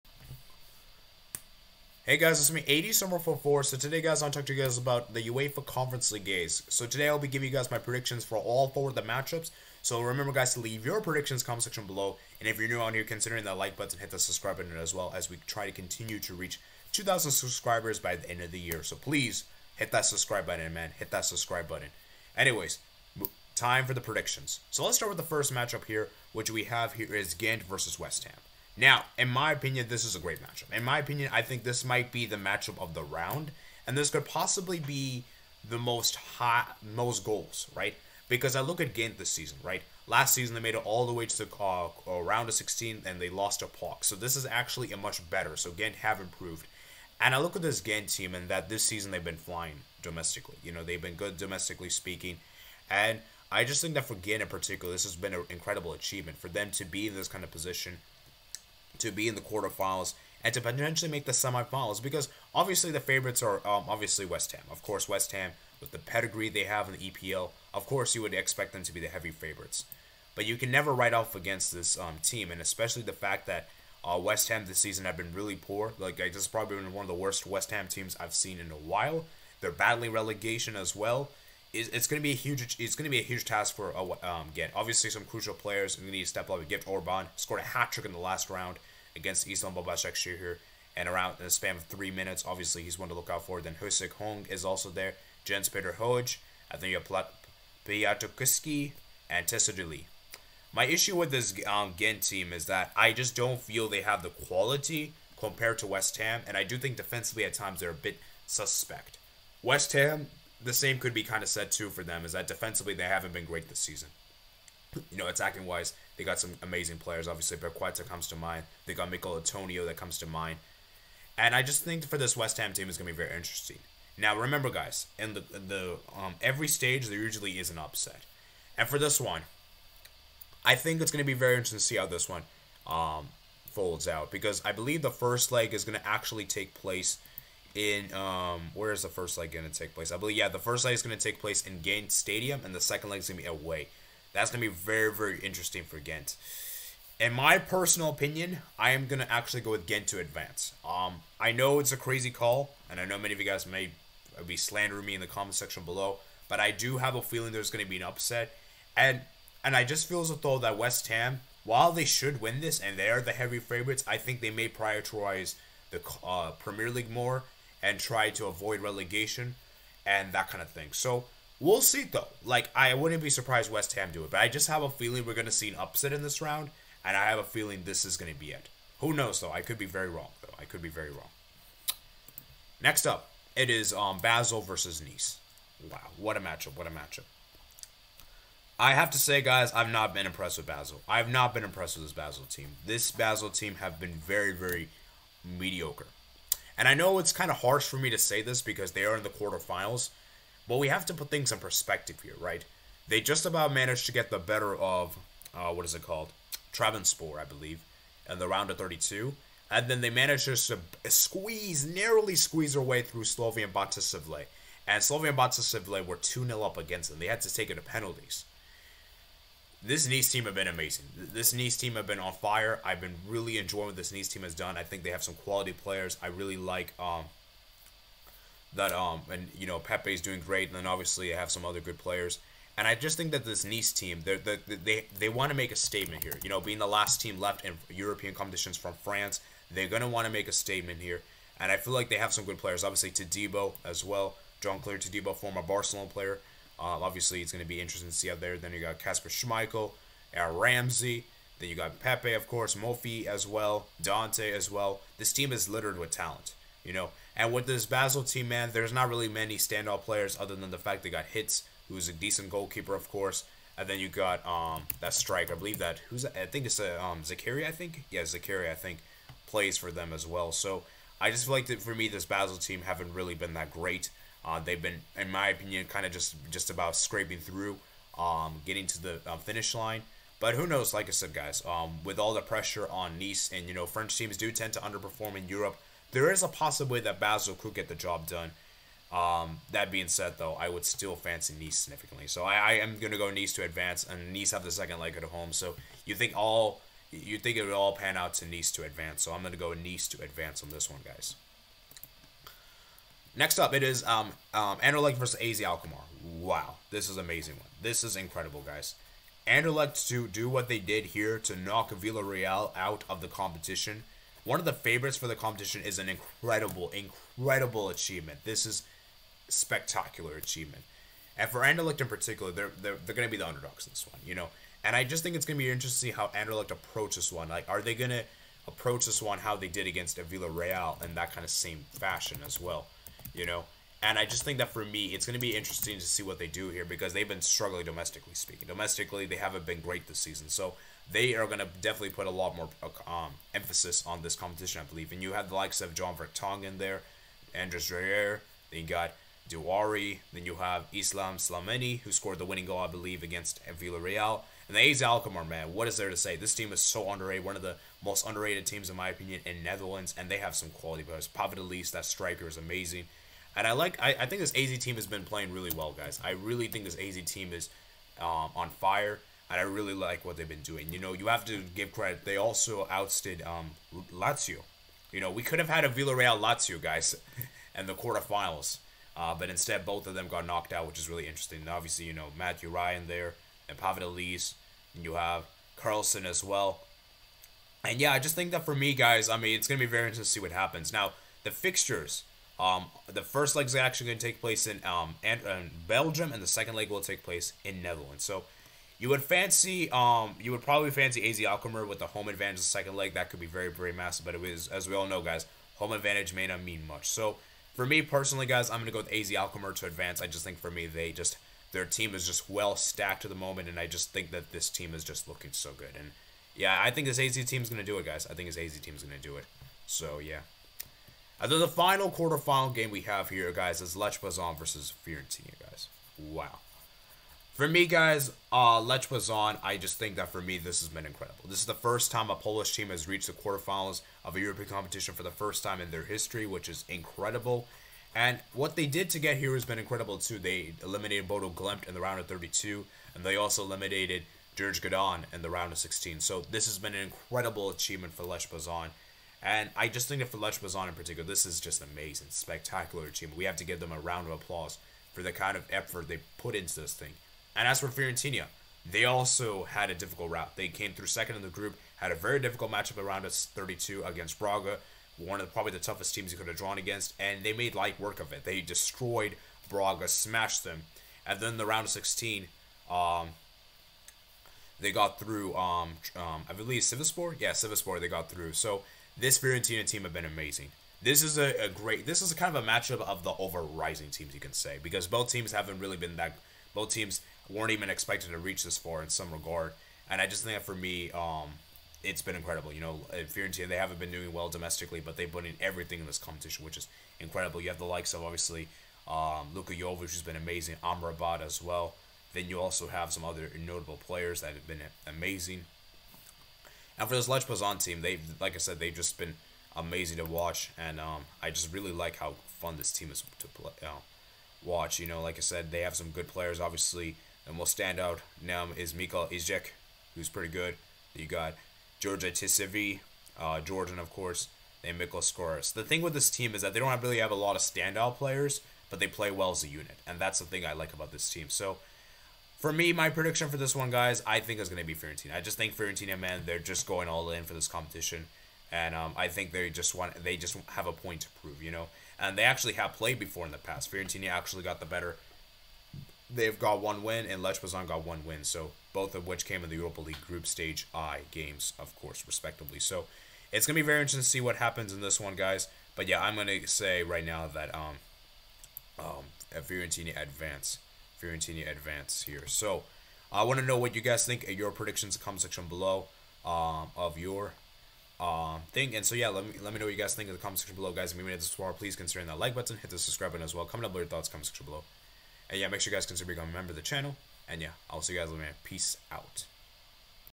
CONFERENCE LEAGUE 22/23 QUARTER-FINAL PREDICTIONS
Timestamps:
0:00 - Intro
0:25 - Gent vs West Ham
3:07 - Basel vs Nice
5:06 - Anderlecht vs AZ
7:04 - Lech vs Fiorentina